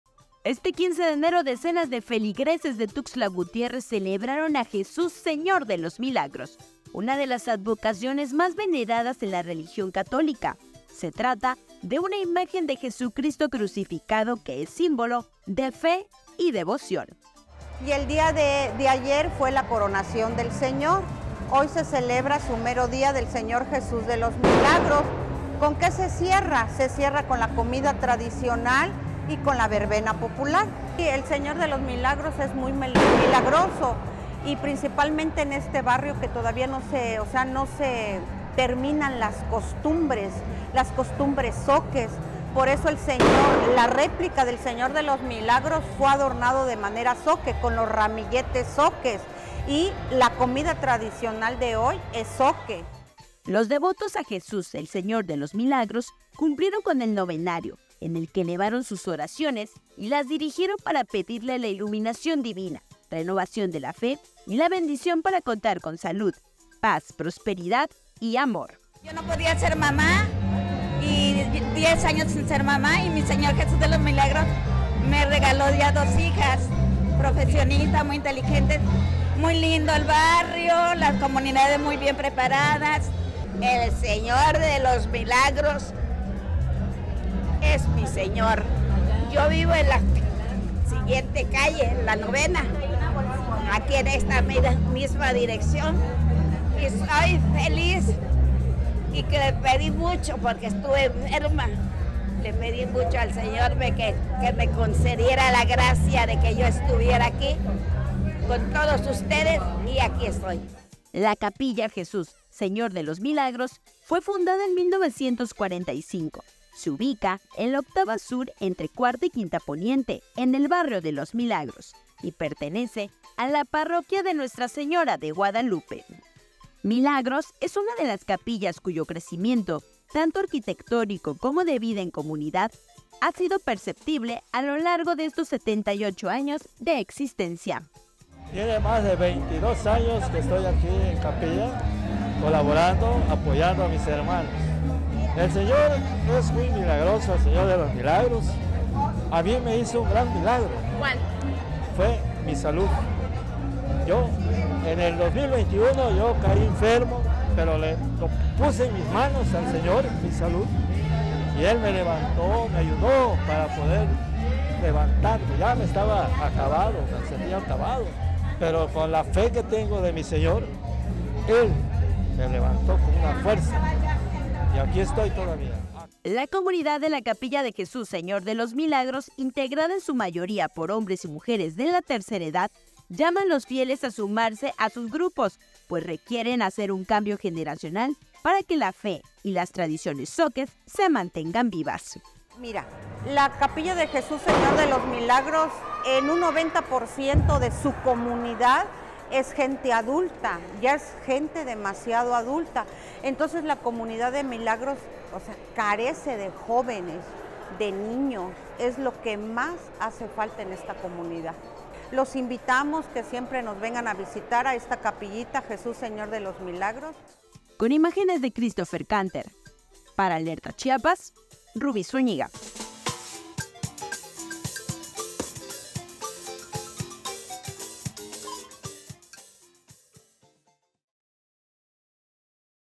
Hoy es el día de Jesús, Señor de los Milagros
Este 15 de enero feligreses celebraron a una de las advocaciones más veneradas en la religión católica, una imagen de Jesucristo crucificado, símbolo de fe y devoción.